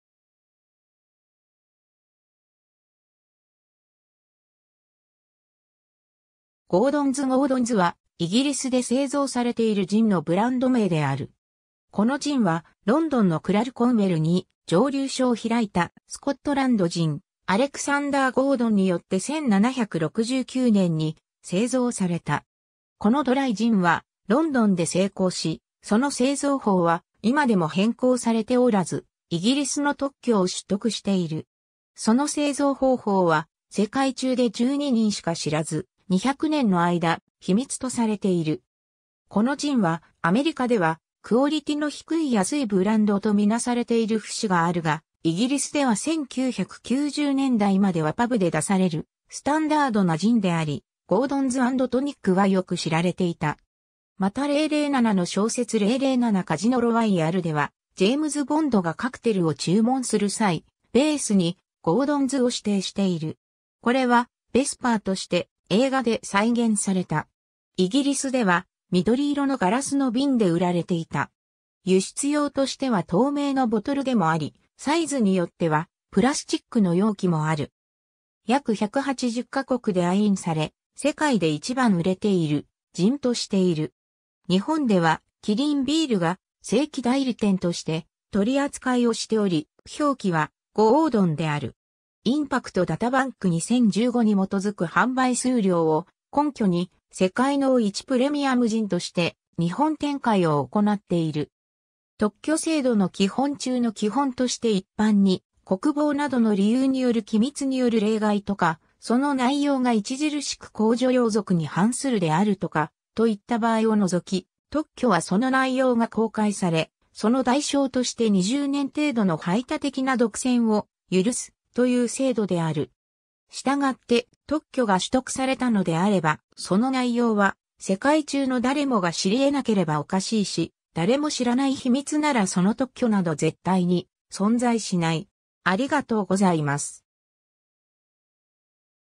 ゴードンズ
ゴードンズ ゴードンズ(Gordon's Gin)は、イギリスで製造されているジンのブランド名である。
 このジンは、ロンドンのClerkenwellに蒸留所を開いたスコットランド人アレクサンダー・ゴードンによって1769年に製造された。
このドライ・ジンはロンドンで成功し、その製造法は今でも変更されておらず、 イギリスの特許を取得している。
 その製造方法は世界中で12人しか知らず、200年の間秘密とされている。
 このジンは、アメリカではクオリティの低い安いブランドと見なされている節があるが、イギリスでは1990年代まではパブで出されるスタンダードなジンであり、"ゴードンズ・アンド・トニック" はよく知られていた。
また007の小説007 カジノ・ロワイヤルではジェームズ・ボンドがカクテルを注文する際、ベースにゴードンズを指定している。
これはヴェスパーとして映画で再現された。
 イギリスでは緑色のガラスのビンで売られていた。
輸出用としては透明のボトルでもあり、サイズによってはプラスチックの容器もある。
 約180ヵ国で愛飲され、世界で一番売れているジンとしている。
 日本では麒麟麦酒(キリンビール)が正規代理店として取扱をしており、表記は『ゴードン』である。
IMPACT DATABANK2015に基づく販売数量を根拠に「世界No1プレミアムジン」として日本展開を行っている。
 ^ 特許制度の基本中の基本として一般に、国防などの理由による機密による例外とか、（通常の特許にはそんなものはまず無いが）その内容が著しく公序良俗に反するであるとか、といった場合を除き、特許はその内容が公開され、その代償として20年程度の排他的な独占を許す、という制度である。
従って、特許が取得されたのであれば、その内容は世界中の誰もが知り得なければおかしいし、誰も知らない秘密ならその特許など絶対に存在しない。